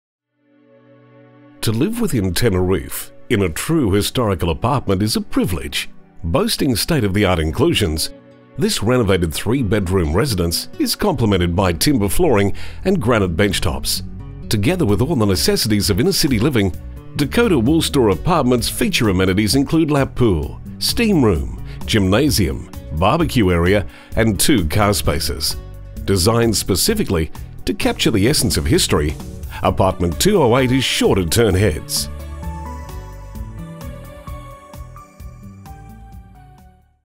208/88 Macquarie Street - Teneriffe (4005) Queensland by Nicholas Given
To live within Teneriffe, in a true historical apartment is a privilege.&nbsp;
&nbsp;
Boasting state-of-the-art inclusions, this renovated 3 bedroom residence is complemented by timber flooring and granite bench tops.&nbsp;
&nbsp;
Together with all the necessities of inner city living, Dakota Woolstore apartments feature amenities include lap pool, steam room, gymnasium, barbecue area and 2 car spaces.&nbsp;
&nbsp;
Designed specifically to capture the essence of history, apartment 208 is sure to turn heads.